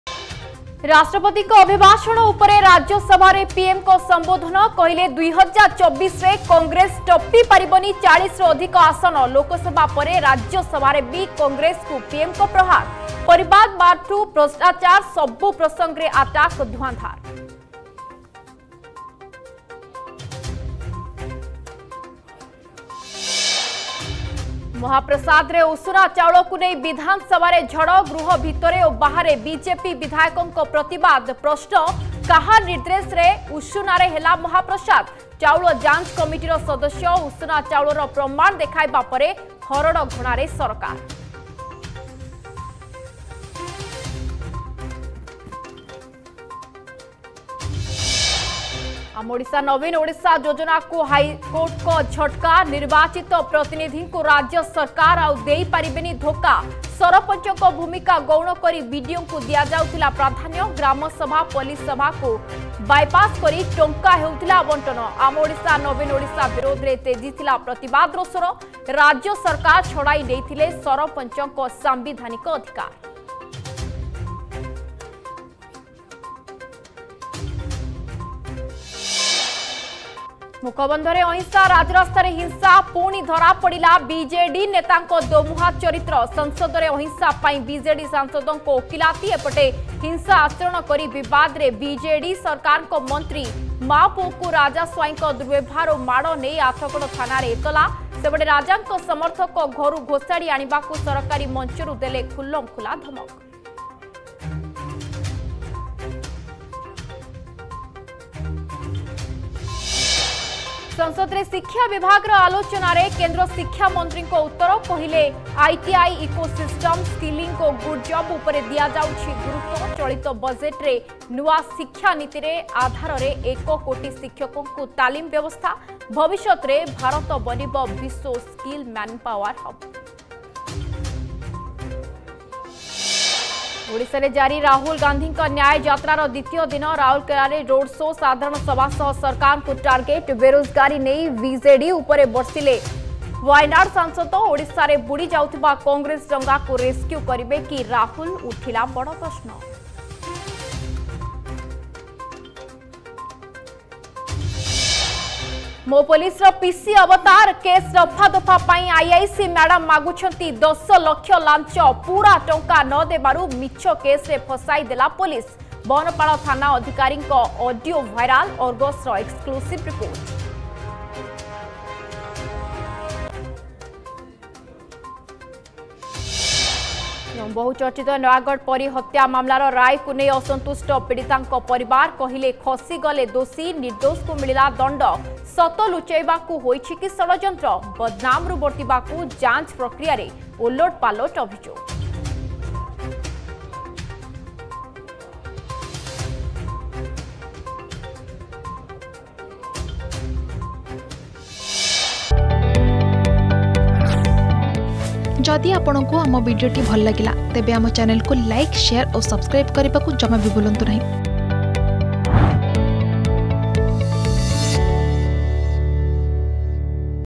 Headlines @ 7 Pm || 7 Feb 2024 || Argus News
+ `କଂଗ୍ରେସ କରିବନି 40 ପାର'
+ ମହାପ୍ରସାଦରେ ଉଷୁନା ଝଡ଼
+ `ଆମ ଓଡ଼ିଶା ନବୀନ ଓଡ଼ିଶା'କୁ ହାଇକୋର୍ଟ ଝଟକା